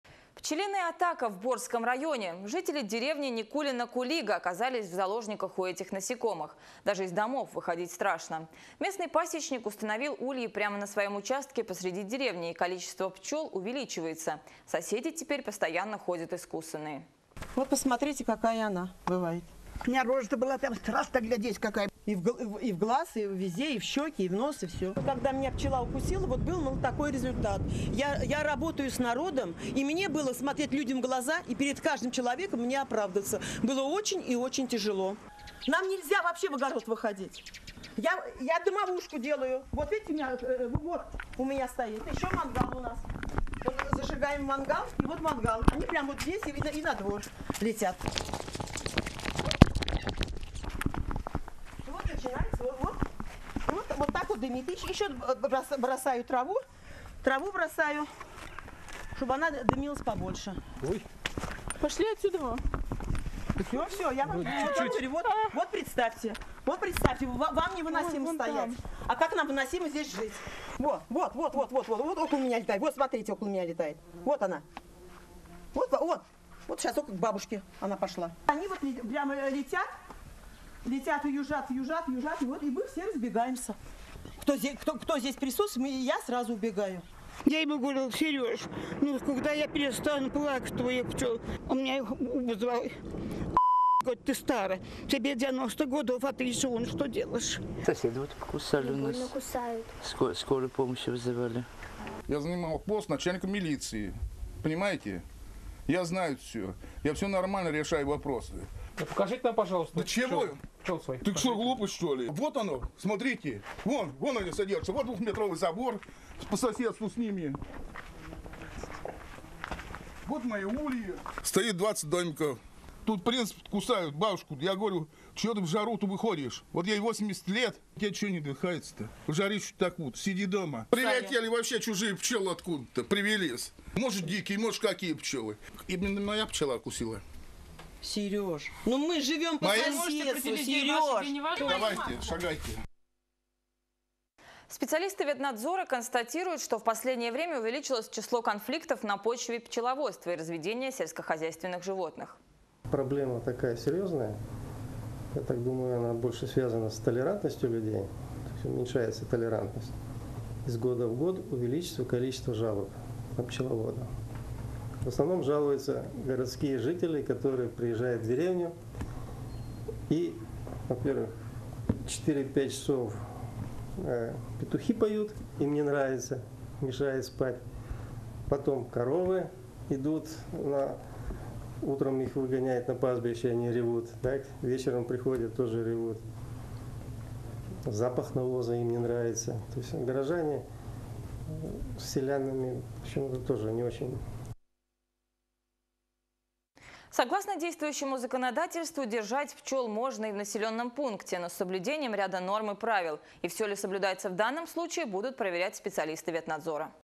Медовые войны - искуссаные садоводы восстали против деревенский пасечников
Пчелиная атака в Борском районе: жители деревни Никулино-Кулига  оказались в заложниках у этих насекомых - даже из домов выходить страшно. Местный пасечник установил ульи прямо на своем участке посреди деревни, и количество пчел постоянно увеличивается. Соседи теперь постоянно ходят искусанные.